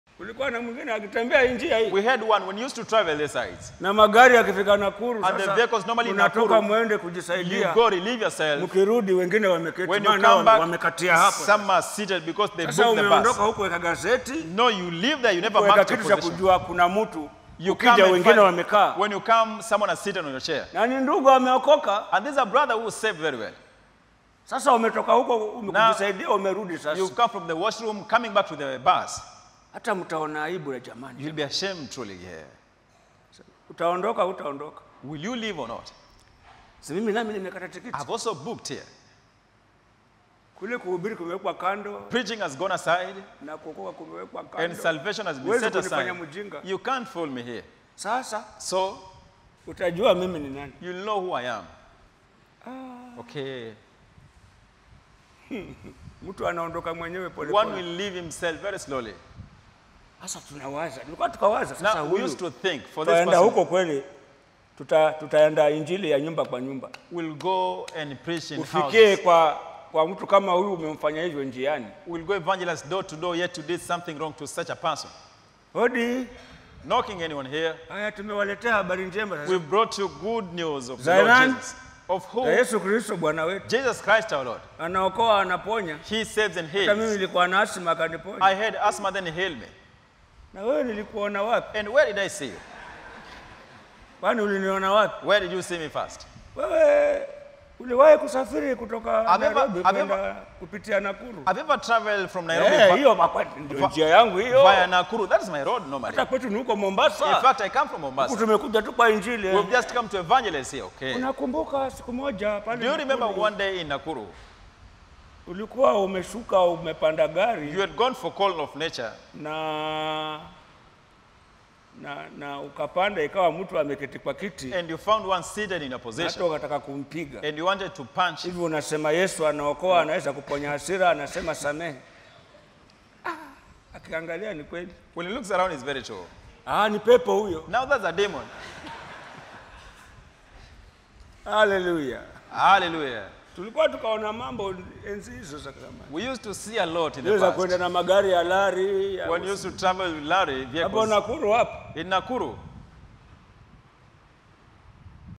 WORD OF THE DAY | NENO LA SIKU
That he no longer should live the rest of his time in the flesh to the lusts of men, but to the will of God.
For the time past of our life may suffice us to have wrought the will of the Gentiles, when we walked in lasciviousness, lusts, excess of wine, revellings, banquetings, and abominable idolatries:
Wherein they think it strange that ye run not with them to the same excess of riot, speaking evil of you: 1Peter4:2-4 [kjv]

Believers redeemed by the blood of the Lord Jesus Christ should always master great courage to forsake doing the will of the flesh. The Servant of God Ismael_Mwadzombo_Rimba cites situations where brethren misrepresent the gospel of Christ unknown to them that they might encounter the very people they offended when carrying out door-to-door witnesses. Always live according to the will of God wherever you go to draw many to Christ.